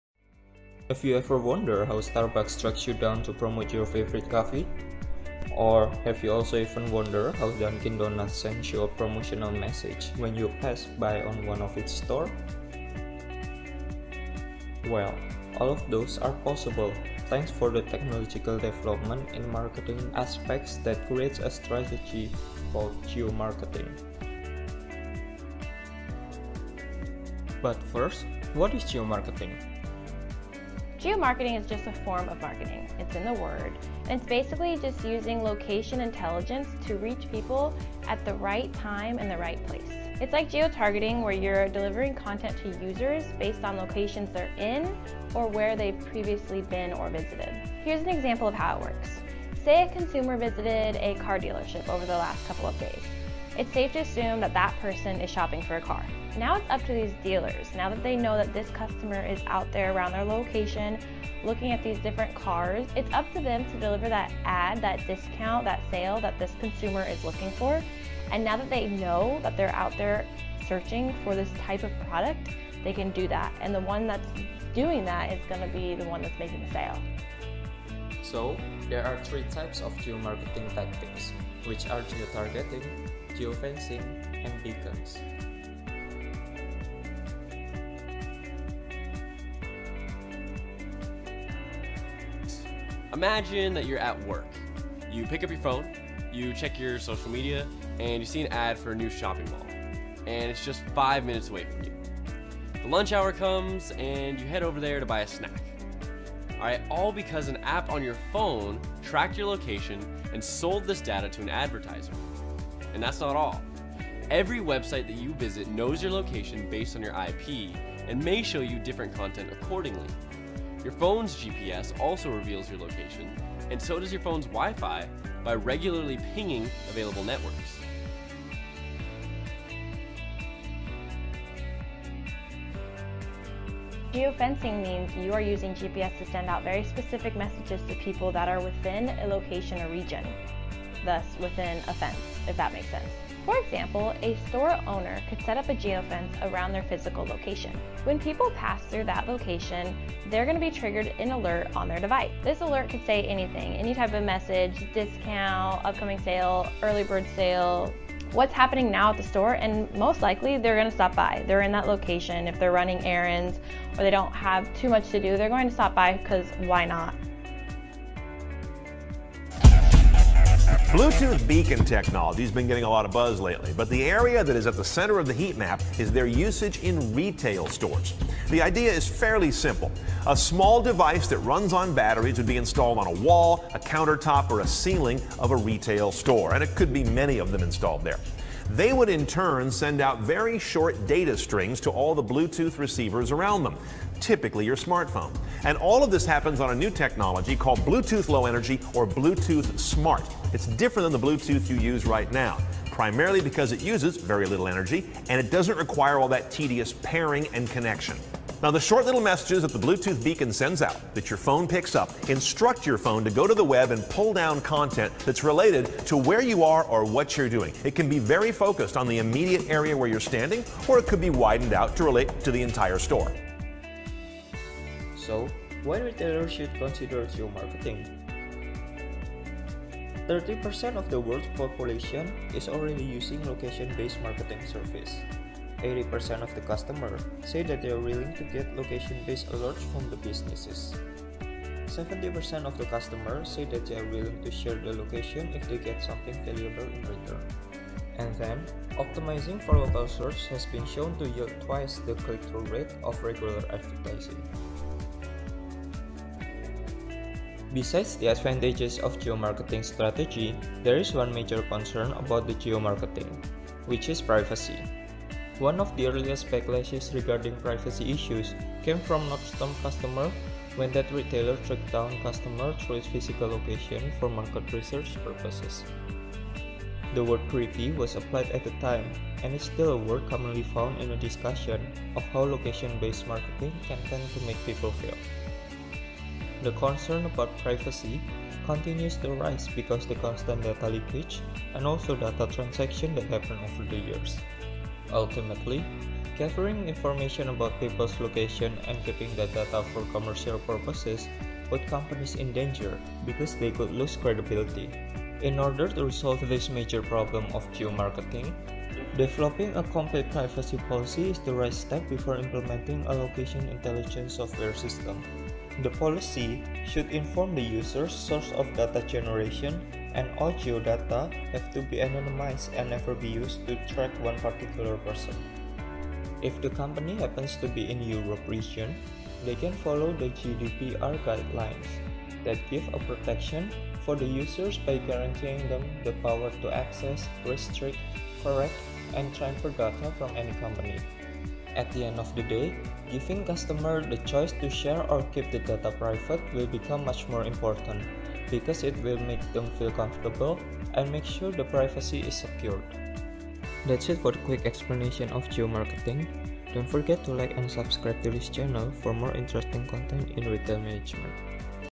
Geomarketing Explained
Geomarketing is the foundation of successful businesses in this age of digitalization and quickly changing markets. Geomarketing involves the geographic analysis and visualization of the company’s data in order to spot trends and relationships that would otherwise go unnoticed. Utilizing digital maps to display the market, customer and company data allows for more effective and efficient decision-making.

[Johanes Immanuel_19018023]